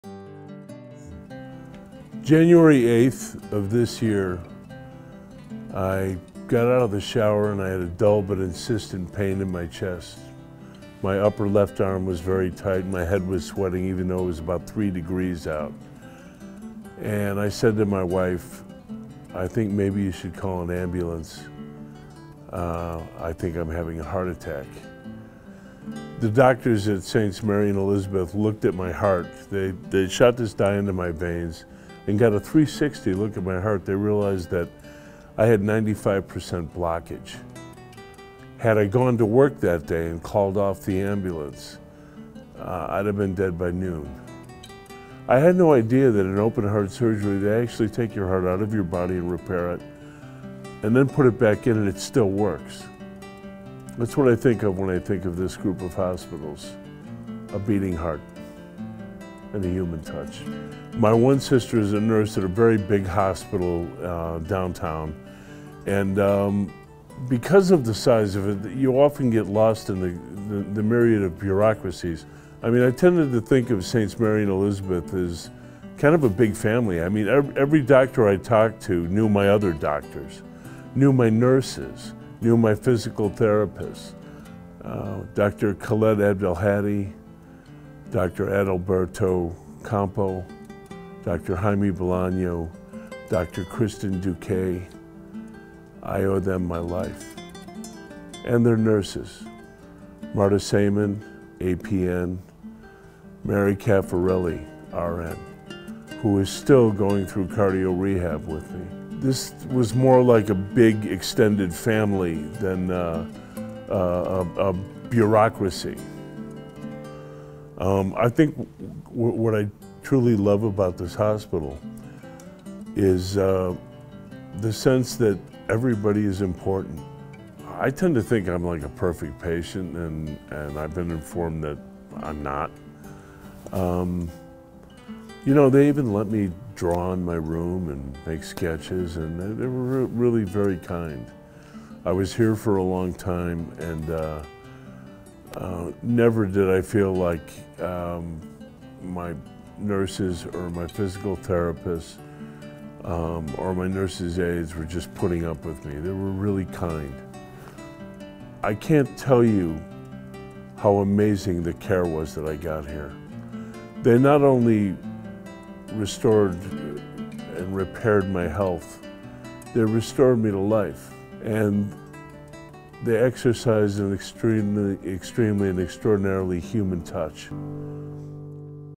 Tony Fitzpatrick: Grateful Patient
This heart attack survivor says, “I think of Presence Health as a beating heart with a human touch. …  It is like a big family, and I owe them my life. … My amazing care not only restored my health, it restored my life – all with a human touch.”